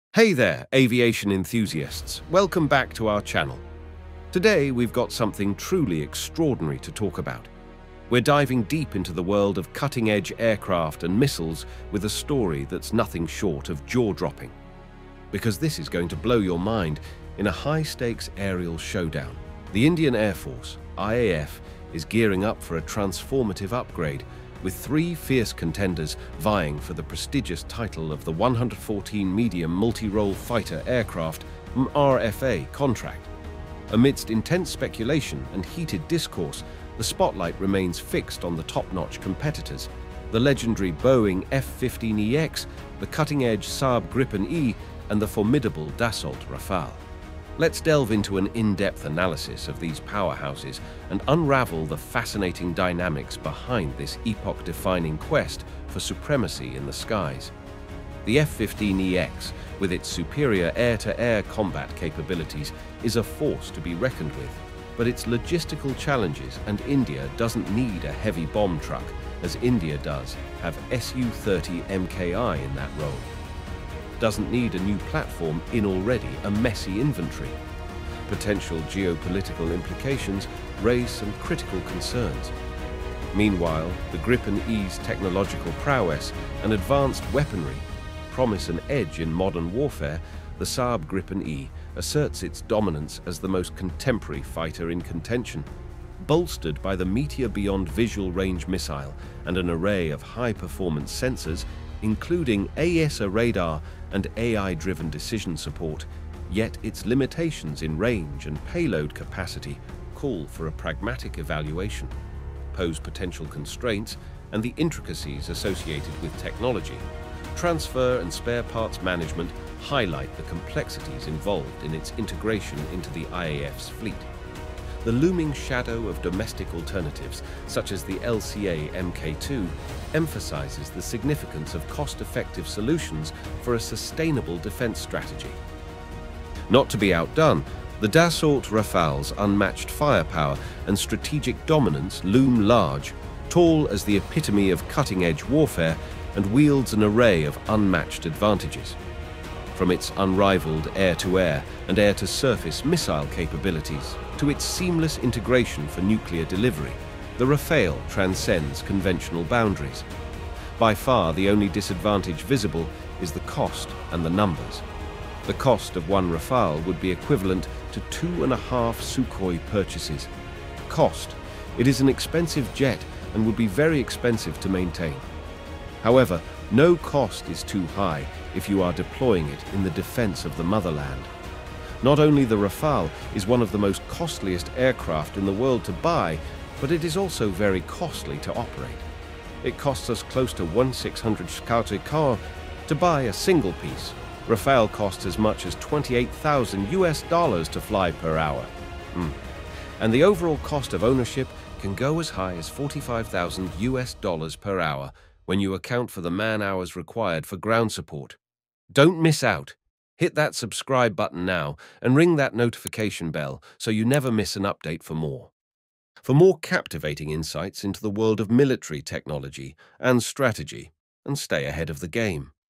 "Sky Warriors: F-15EX vs Gripen-E vs Rafale
"Uncover the pulse-quickening race for India's Next-Gen Fighter Jet Dominance! Join us in dissecting the strengths of F15EX, GripenE, and Rafalefighter in a gripping analysis. 🛩️ IAFUpgrades AerialDominance"